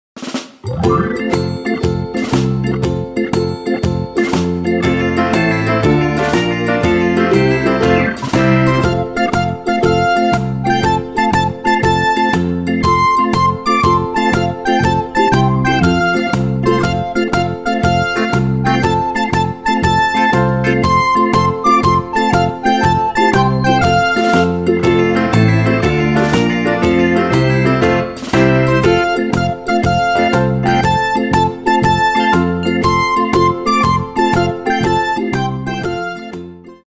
TRT Çocuk Çizgi Film Eymen ile Çimen Çizim ve Boyama | Nasıl Çizilir? | Cartoon
TRT Çocuk Çizgi Film Eymen ile Çimen nasıl çizilir?TRT Çocuk Çizgi Film karakterleri çizimleri.